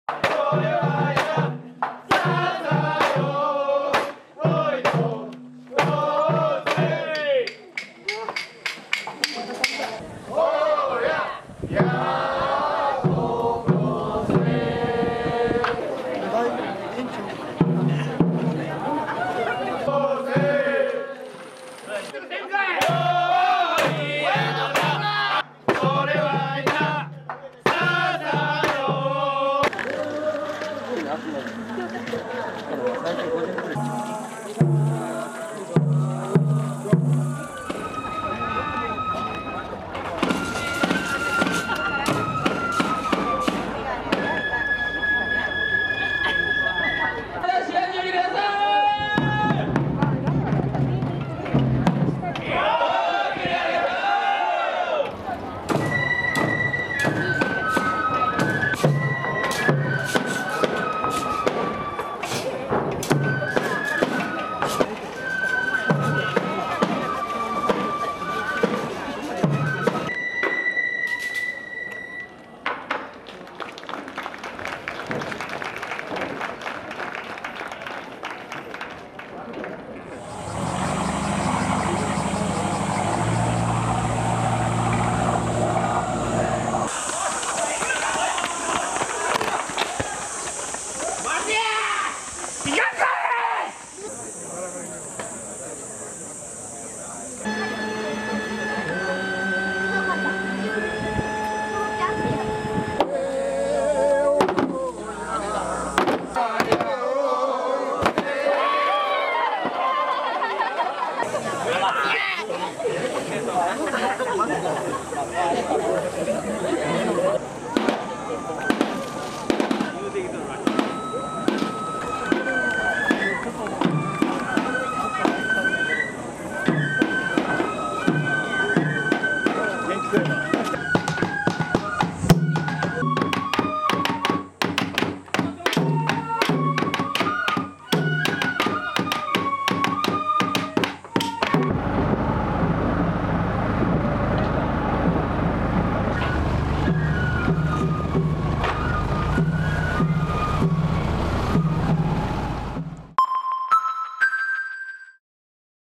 天神祭、夏の海上に活気 駆ける檀尻船、獅子舞奉納 姫路・家島神社
播磨灘に浮かぶ家島に本格的な夏の到来を告げる家島神社（姫路市家島町宮）の例祭「天神祭」が２２、２３日、神社周辺で行われた。きらびやかな「檀尻（だんじり）船」の上で獅子舞が披露され、提灯（ちょうちん）行列の練り歩きや打ち上げ花火もあり、島内外からの見物人でにぎわった。